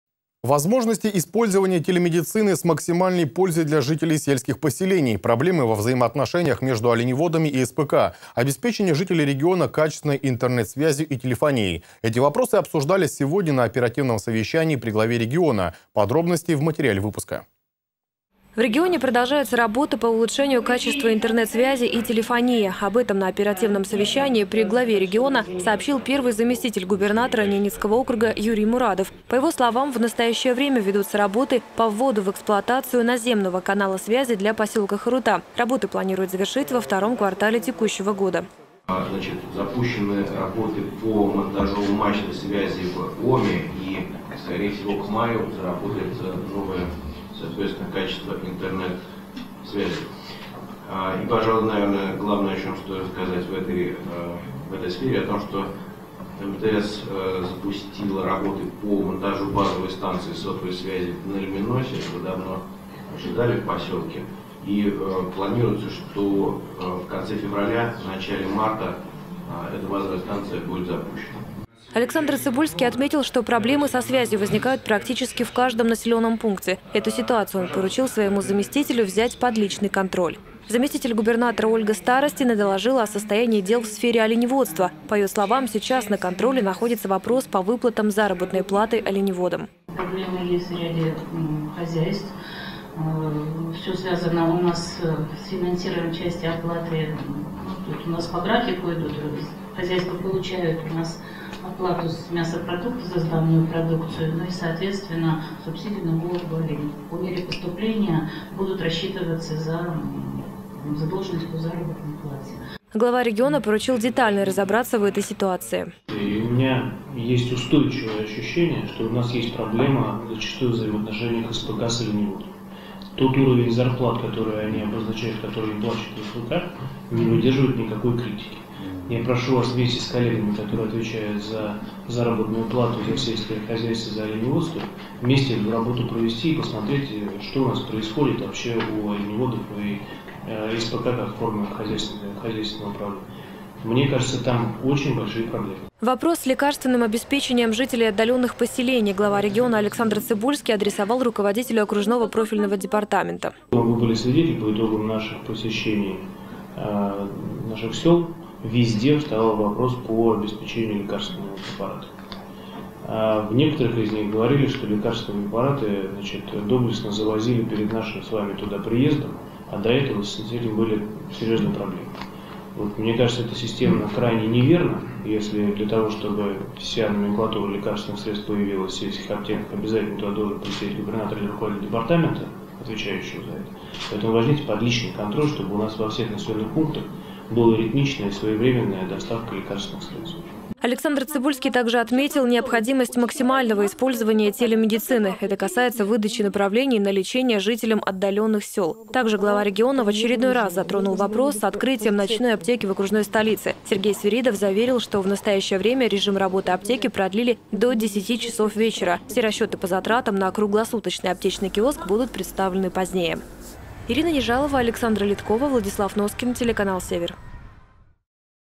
Возможности использования телемедицины с максимальной пользой обсудили в НАО
Возможности использования телемедицины с максимальной пользой для жителей сельских поселений, проблемы во взаимоотношениях между оленеводами и СПК, обеспечение жителей региона качественной интернет-связью и телефонией. Эти вопросы обсуждались сегодня на оперативном совещании при главе региона.